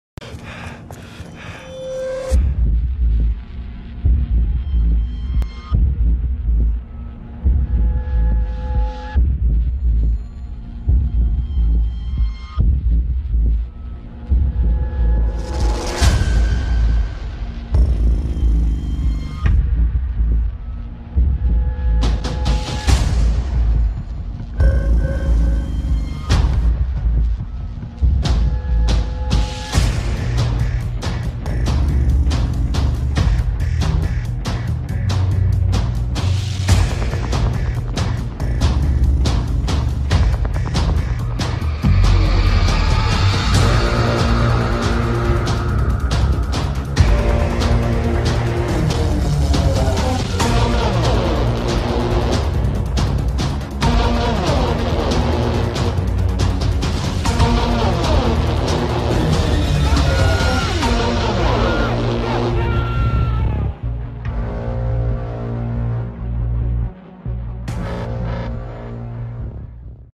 AroundOurSea Promo Oficial by Deus Soul
Que onda SnipersMéxico! Aqui la promo de este clan que va empezando pasen a checar su contenido  esperamos que dejen su like & comentario! Saludos :D